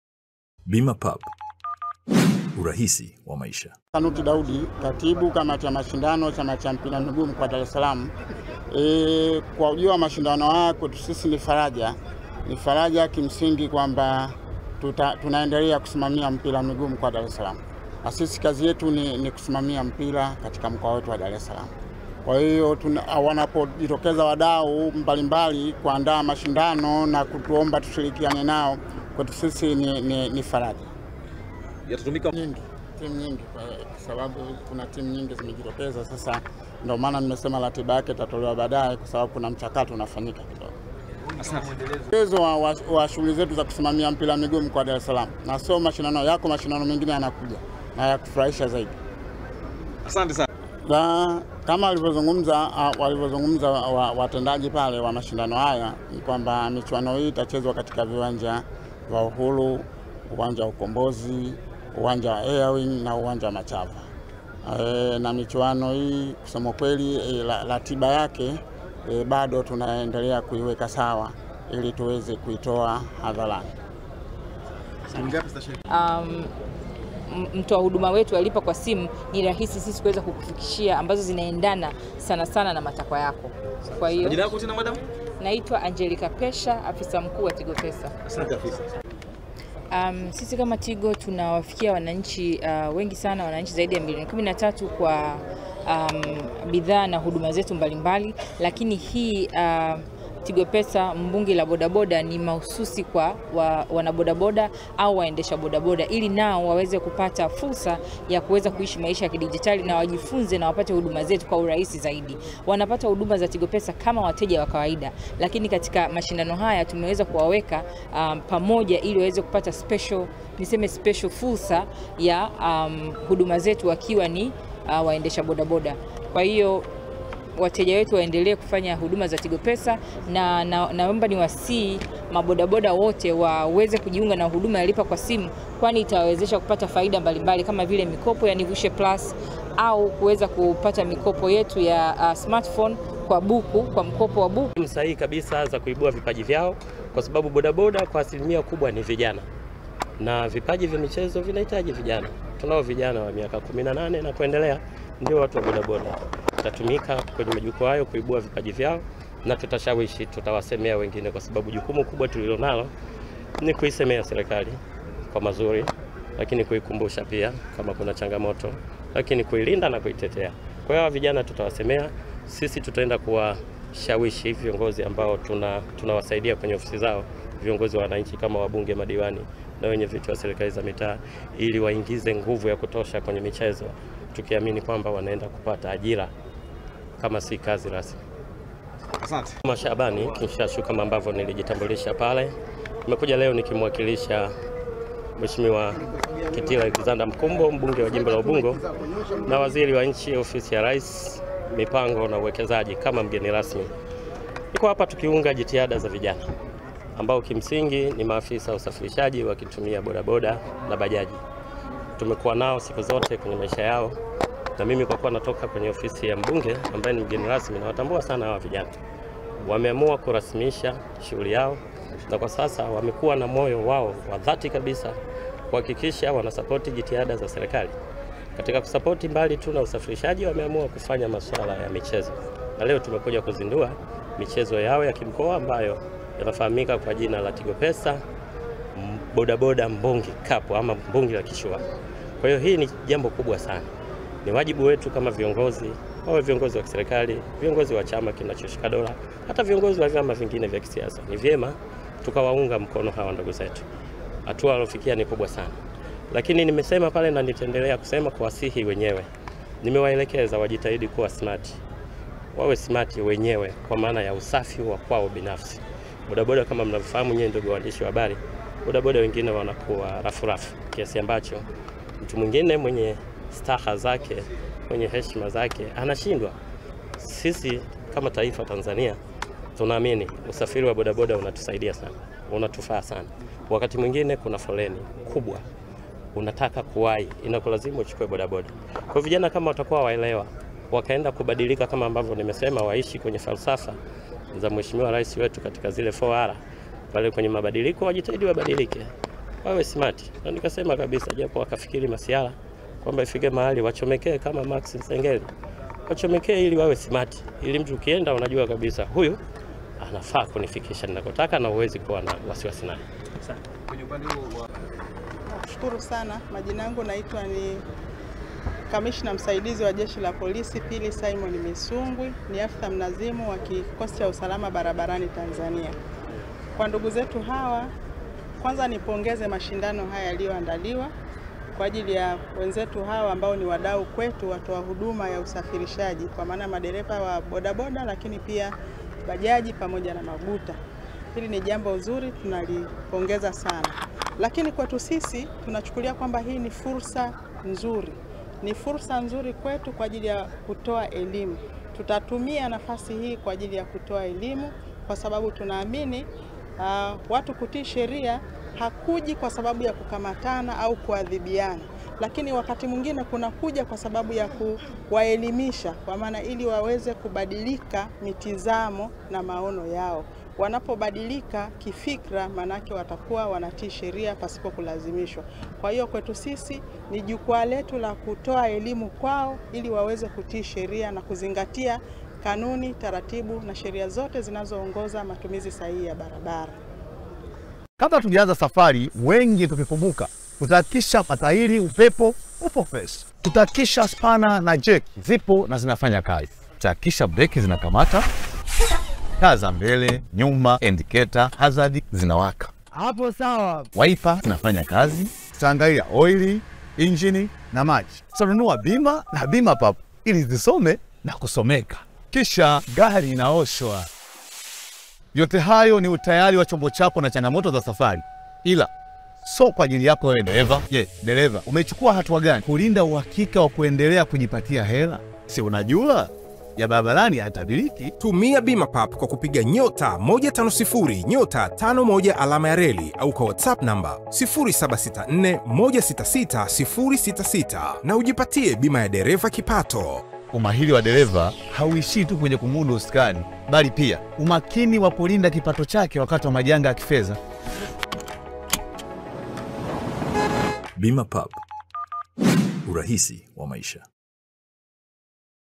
BODABODA IGENI KAMA MAX NZENGELI, VUMBI LITATIMKA, BODABODA CUP YAZINDULIWA RASMI...
Kwenye uzinduzi wa mashindano ya Bodaboda mkoa wa Dar es salaam Bodaboda Cup muwakilisi wa Mgeni rasmi Shabani Kinshashu amewataka Bodaboda kuwa Smart wakiwa kwenye kazi yao kama mchezaji wa Yanga Max Nzengeli kuchomekea ili kuiheshimisha kazi yao.